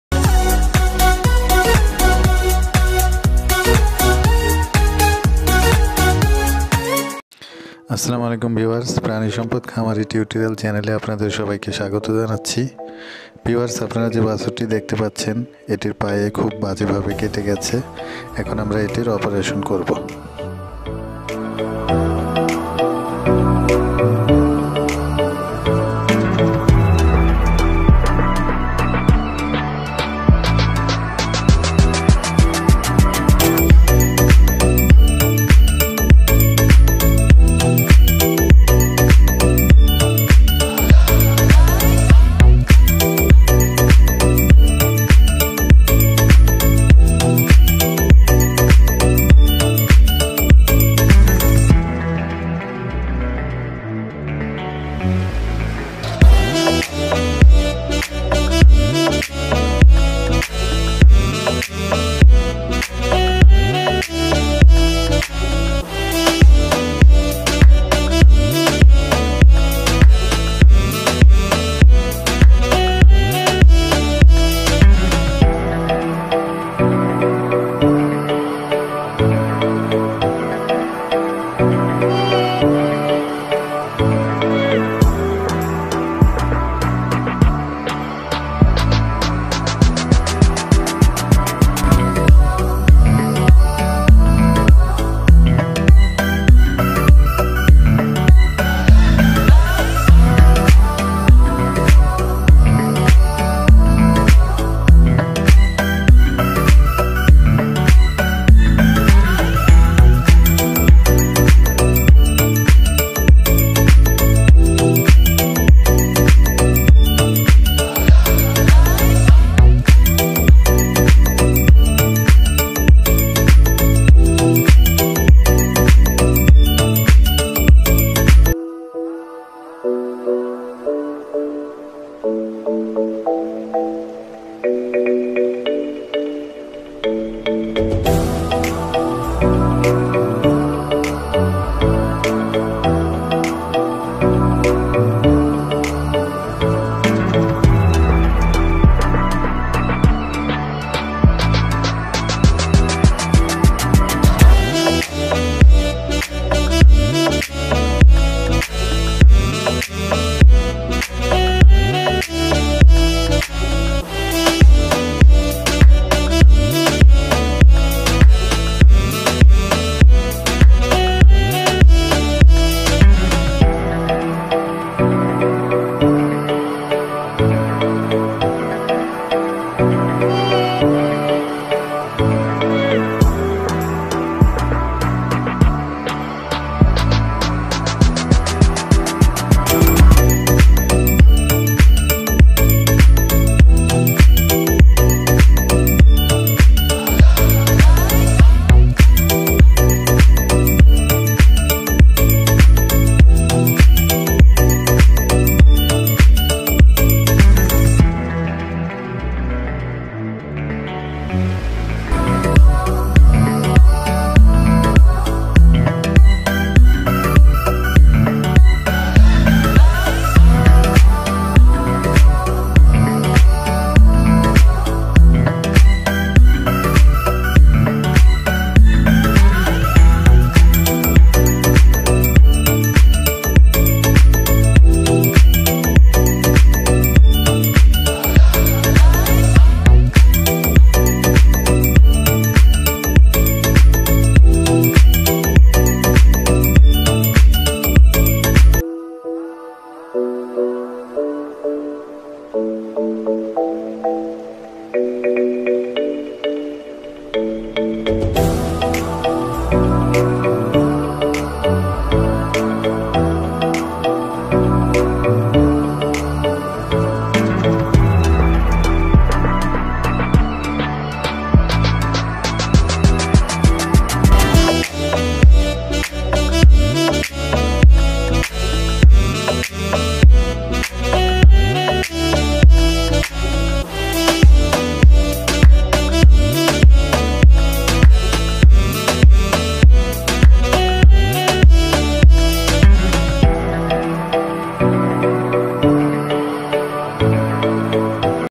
বাছুরের কাটা পা অপারেশন করা হলো
treatment of a wound of a calf
treatment of a calf